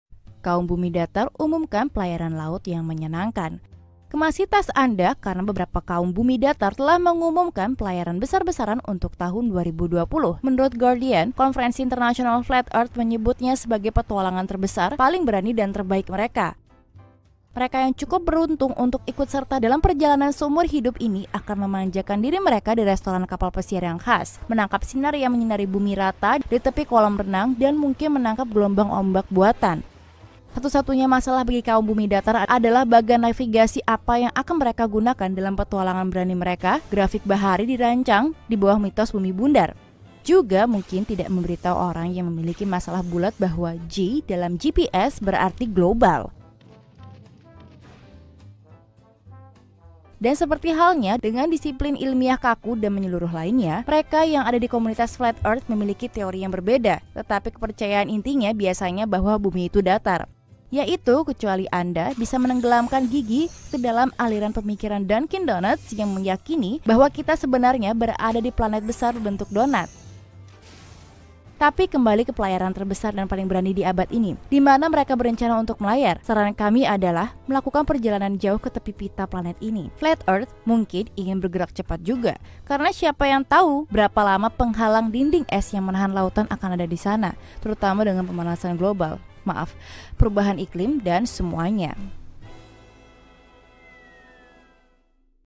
Flat Earth goes to Antarctica
⛵ BUMI DATAR — Kemasi tas Anda karena beberapa Flat Earther yang bagus telah mengumumkan pelayaran besar-besaran pada tahun 2020, menurut The Guardian. Konferensi Internasional Flat Earth menyebutnya sebagai "petualangan terbesar, paling berani, terbaik mereka."
Follow akun @101kfe.id ✅
Quote berbobot hanya di Instagram kami.